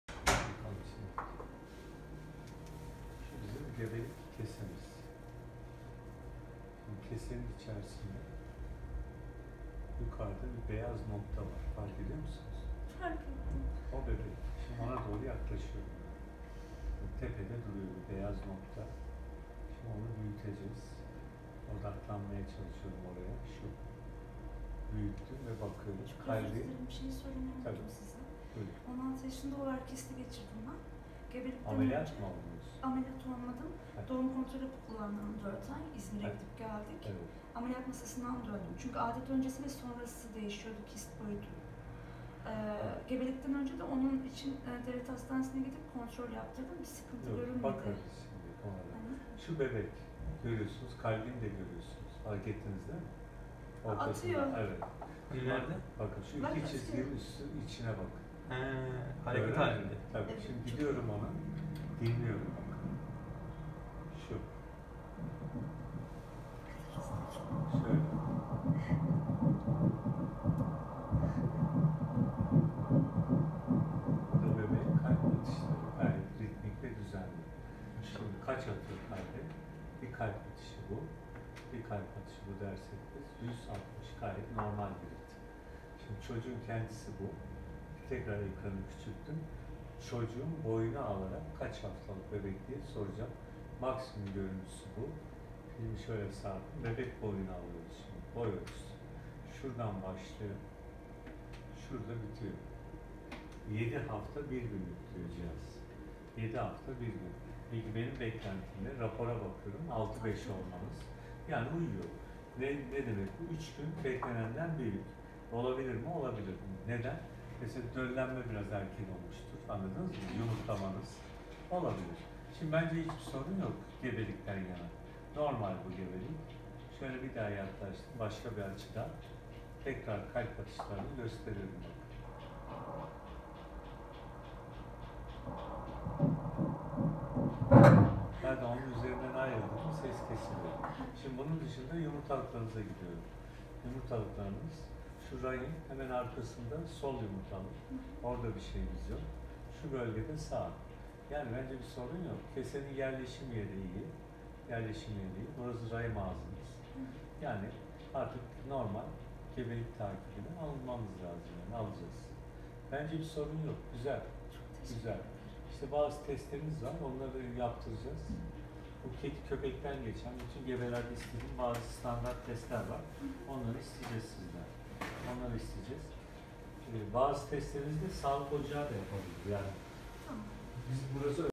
6 Haftalık Gebelik Ultrason Görüntüleri
6 haftalık gebelik ultrason görüntüsü ve anne karnındaki durumu. Hamilelik takibi. Hamilelikte 6. haftada bebeğin kalp atışı ve gelişimi hakkında ultrasondaki izlenimleri.